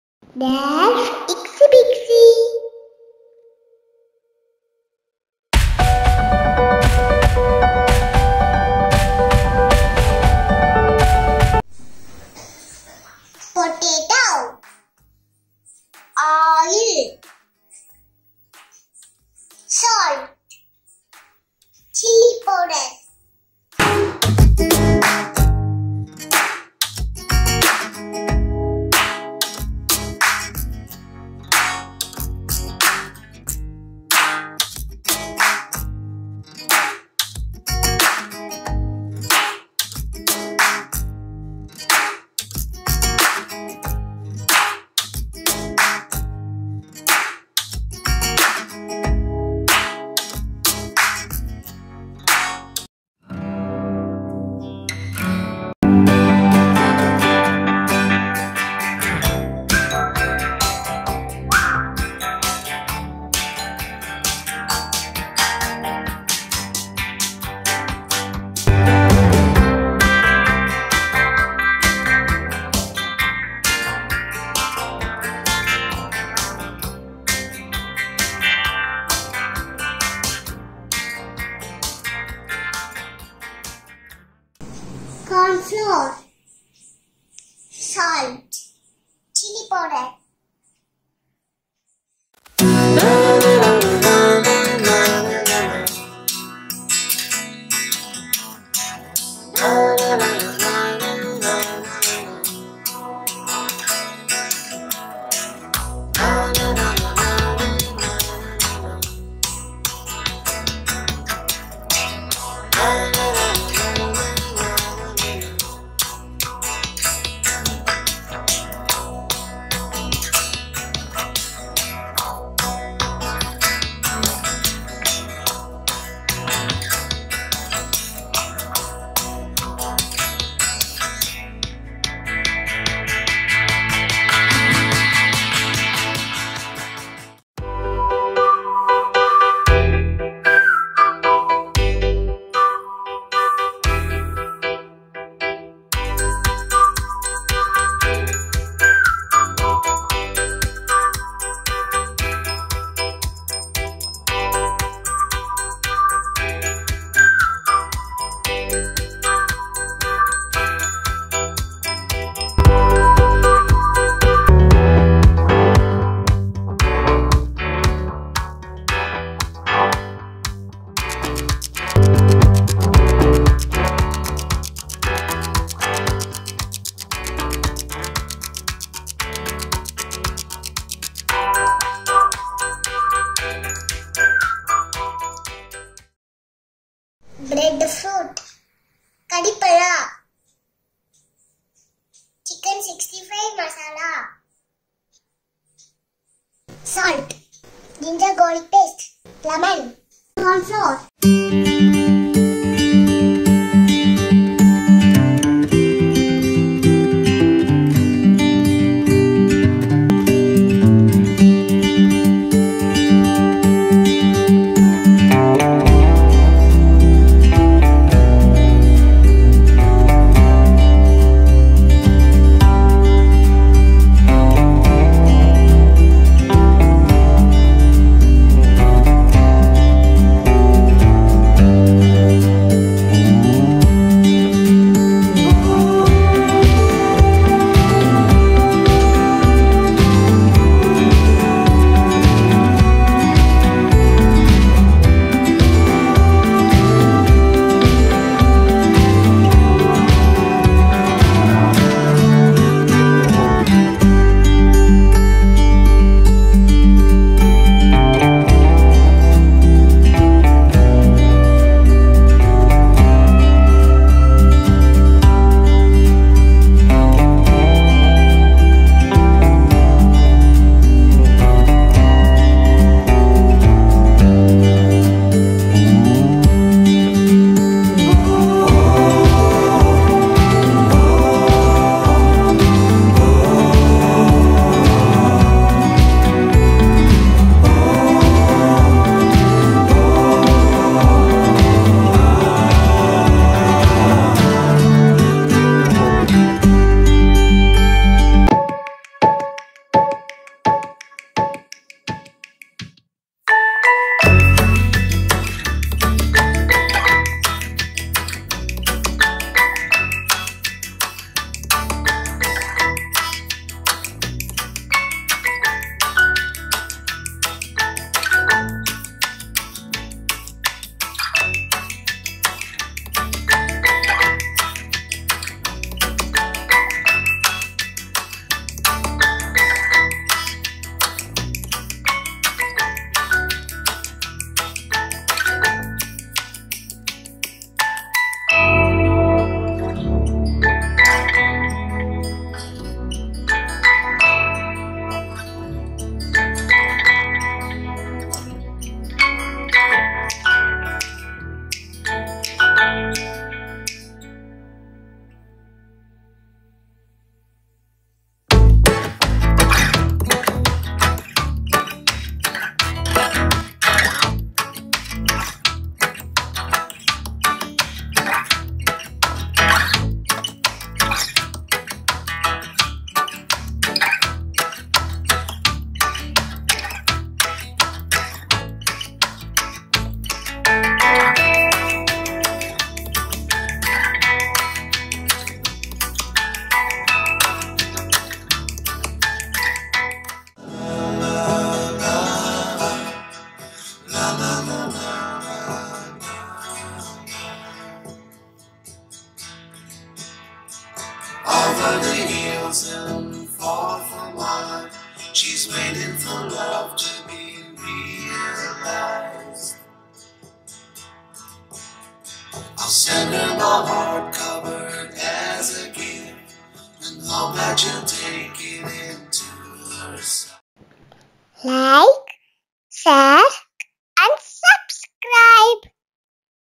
Chips Recipe video | Veg65 | Nendran Banana Chips | Potato Chips |Finger Chips | Breadfruit - கறிபலா
In this video, we will see how to make Potato Chips, Nendran Banana Chips and Karipala 65.